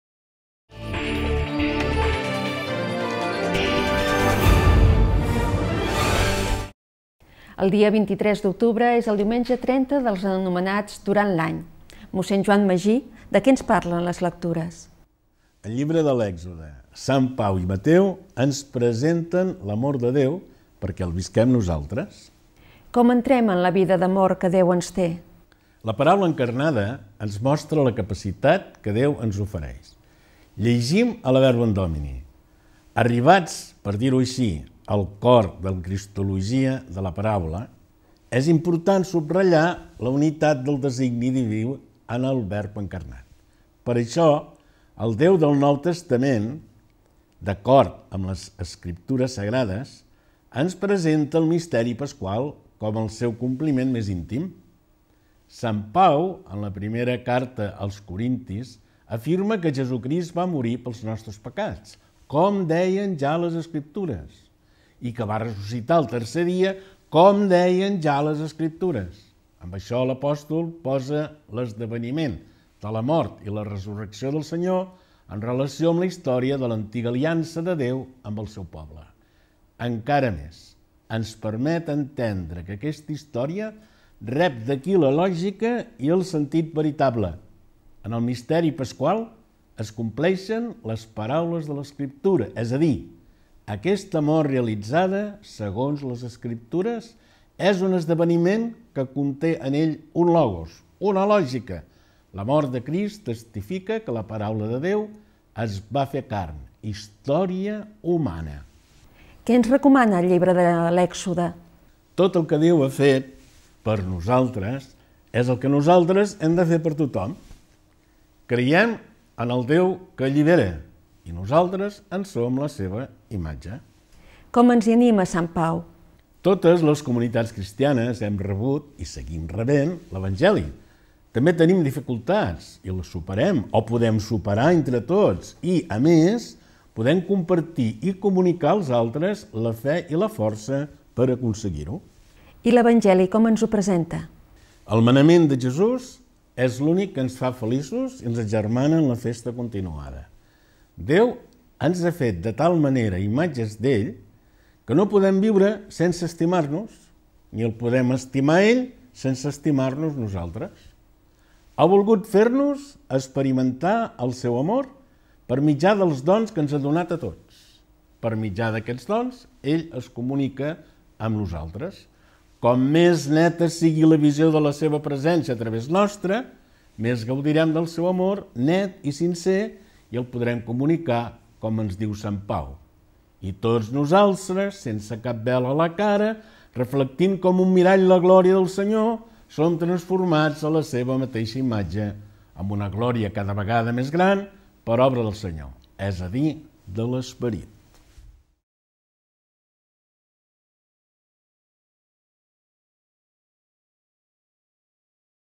«La bona nova», DIUMENGE XXX DURANT L´ANY (A)
Comentari de l'evangeli d'aquest diumenge dia 23, a càrrec de Mn. Joan Magí, Director de Pastoral Bíblica i president de l'Associació Bíblica de Catalunya